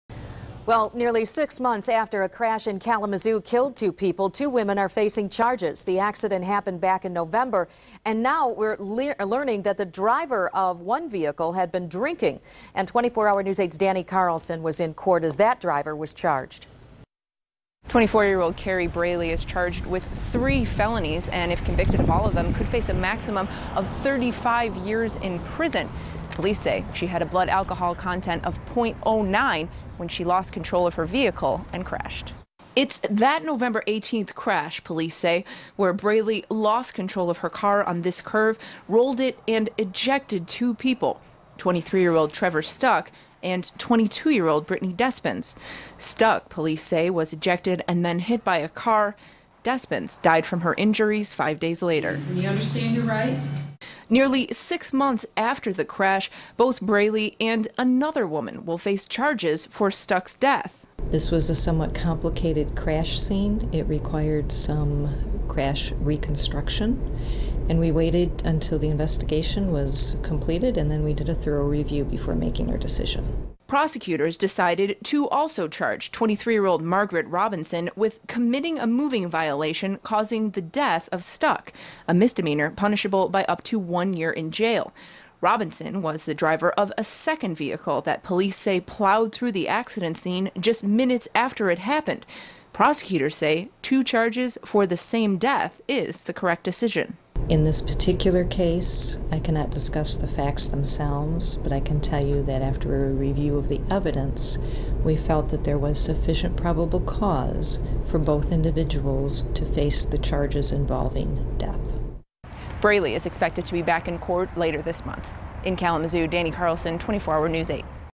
Charges in fatal, chain-reaction wreck
Charges have been issued six months after a deadly car crash that resulted in the deaths of two off-duty ambulance company employees.